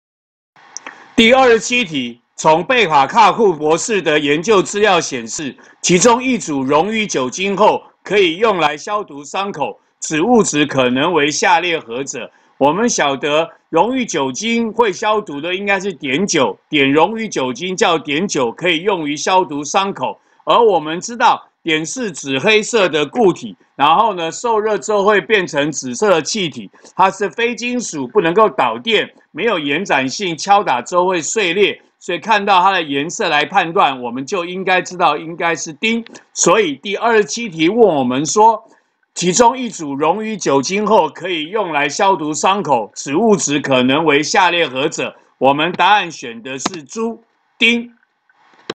105學年上8年級理化三段第27題
25～28題為題組。
給四種物質測試結果表，問何者溶於酒精可消毒。